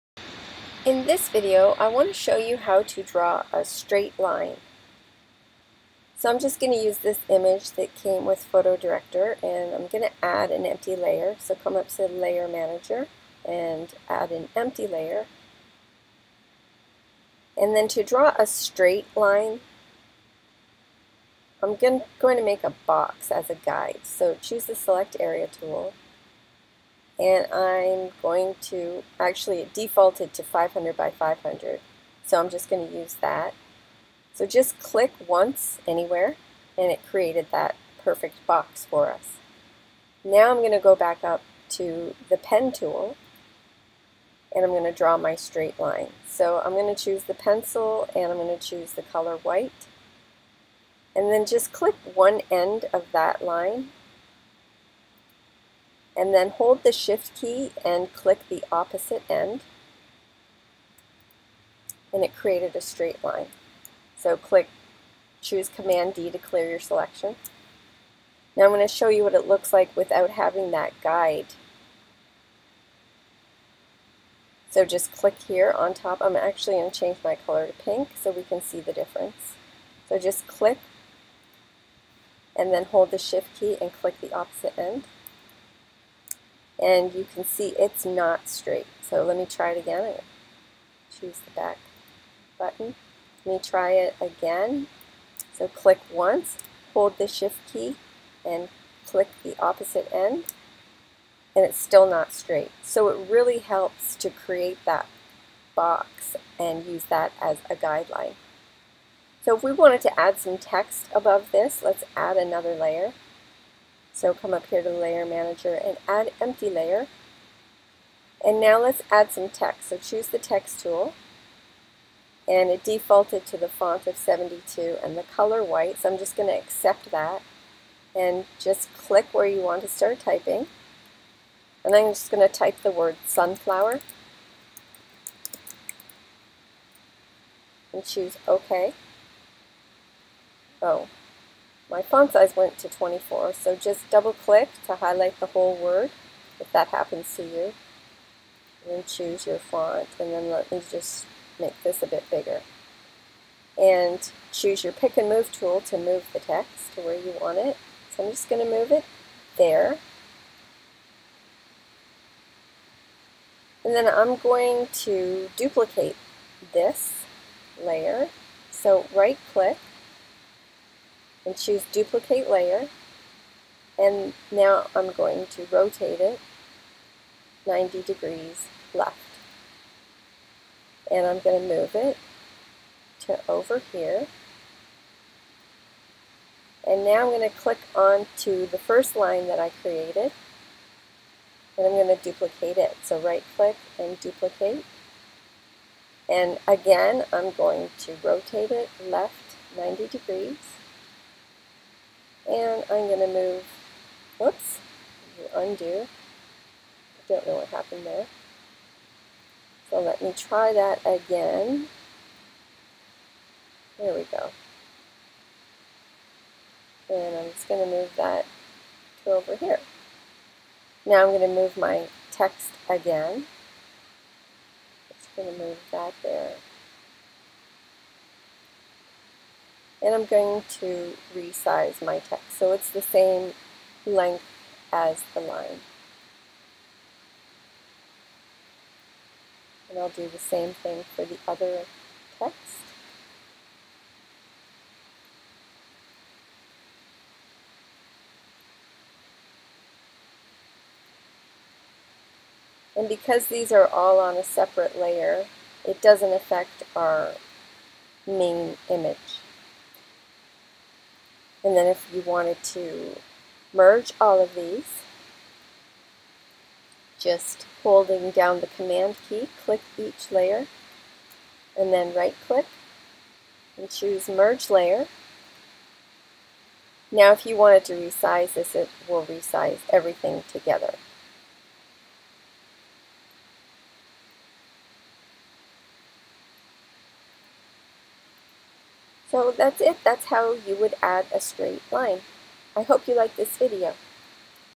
PhotoDirector Tips - How to Draw a Straight Line!
PhotoDirector
In this video I show you the BEST way to draw a straight line using Layers. This method is the most effective way to ensure your lines will always be straight.  And by drawing on separate layers, this ensures that your main photo will remain in tact.  By creating separate layers, we can turn them on or off as we choose.